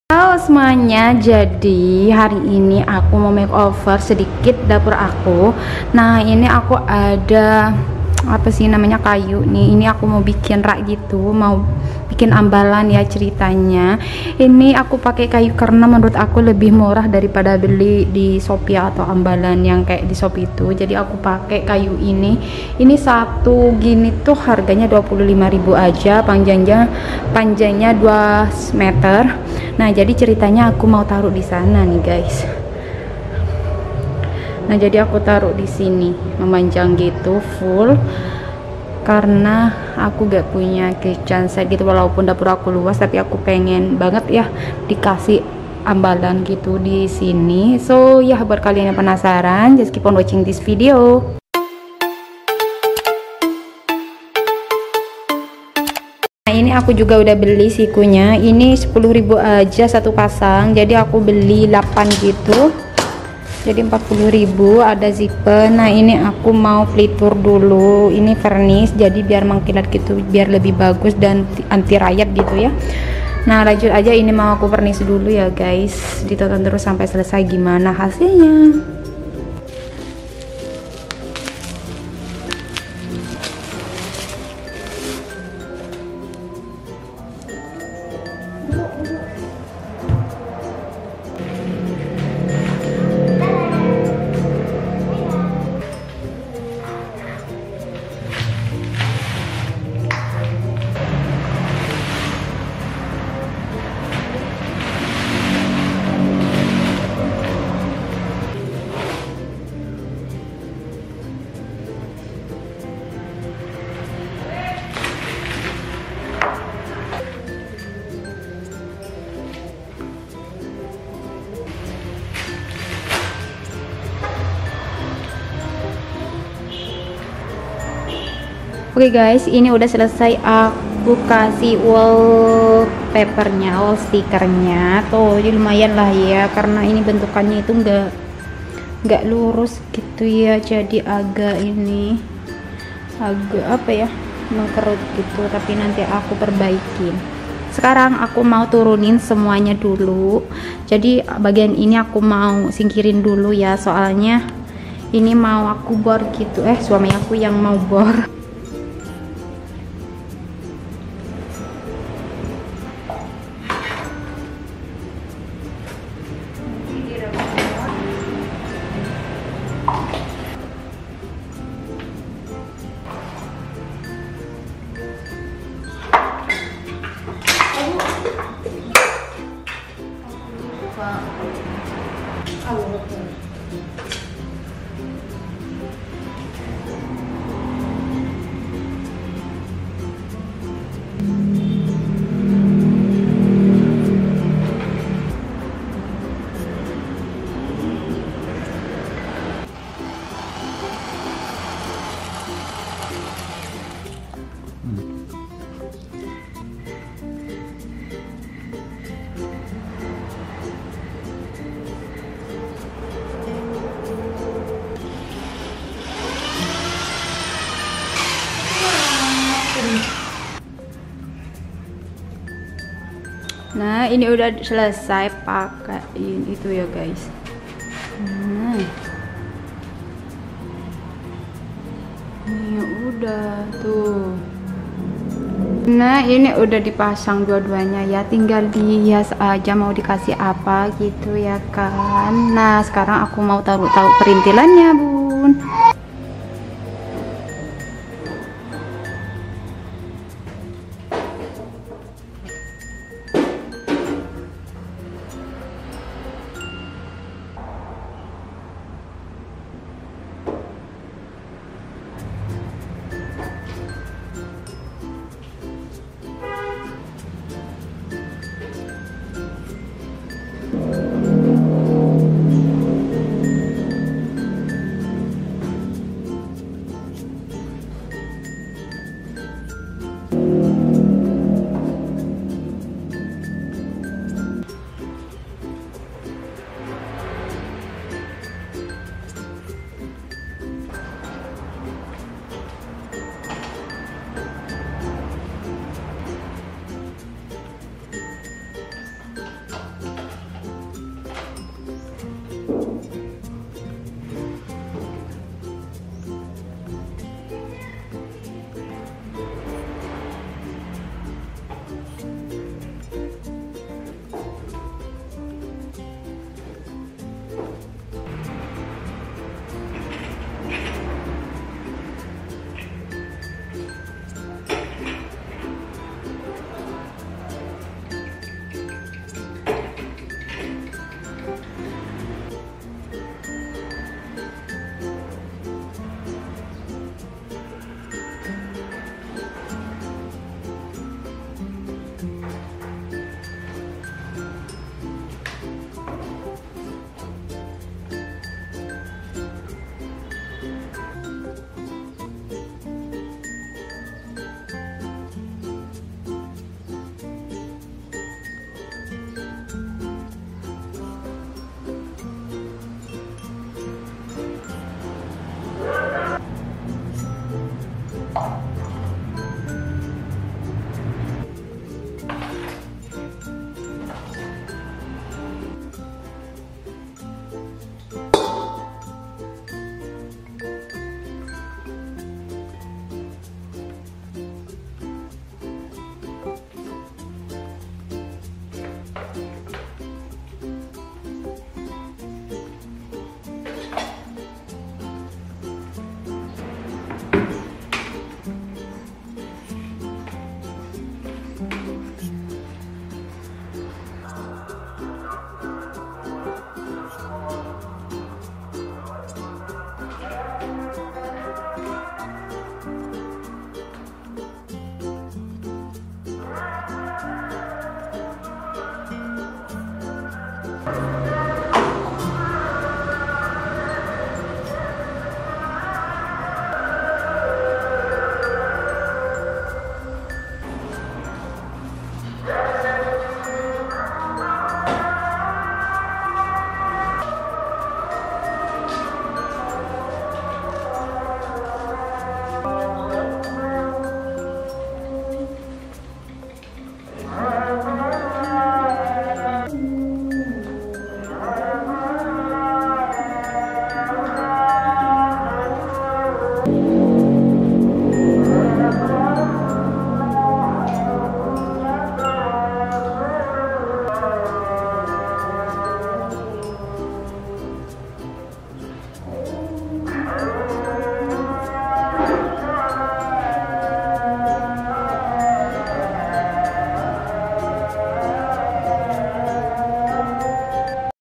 MAKEOVER & DECOR DAPUR PINK | Membuat Rak Dinding Di Dapur
Halo semuanya, kali ini aku mau sedikit makeover dapur ya karena udah bosen jadi mau sedikit dirubah

5. Daun rambat Jintao artificial
9. Hiasan nama Laundry room

Produk:
Rak pengiris piring